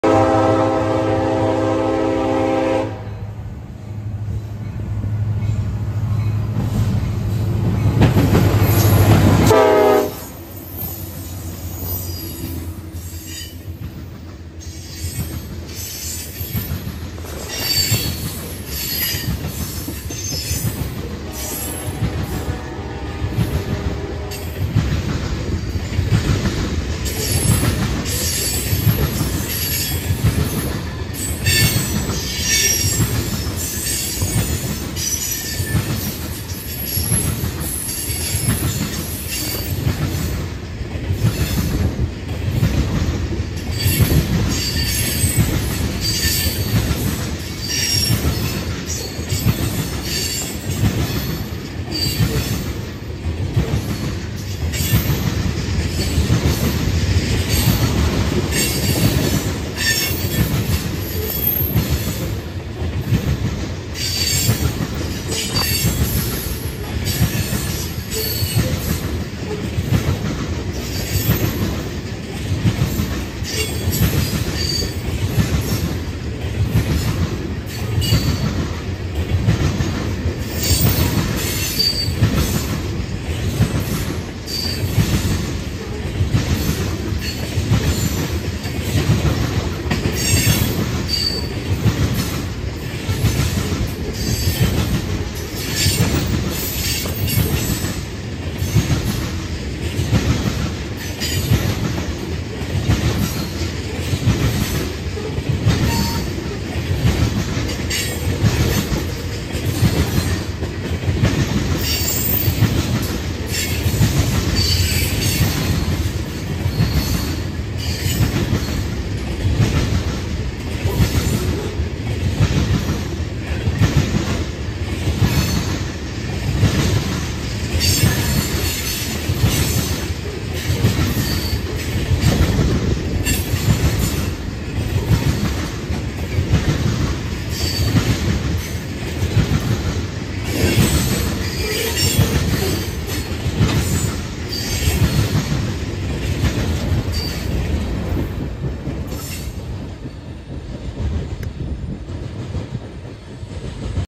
NS tanker train with a surprise!
This NS train gave me a surprise that I have never seen before!
The Union Pacific Employee Assistance Program engine (UP 8388) I had no idea this was even coming until it snuck up on me!

(Copyrights applied)